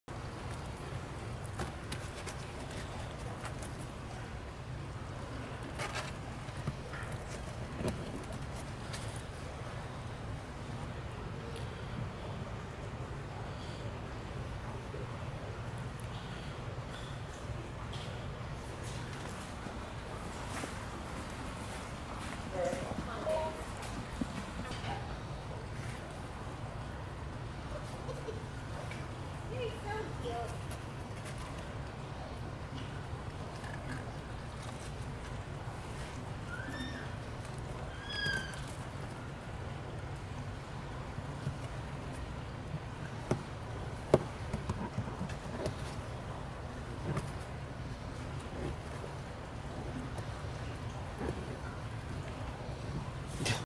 Lincoln is fluffy, squeaky and a keystone species
Sea otter pup Lincoln is having a very fluffy day.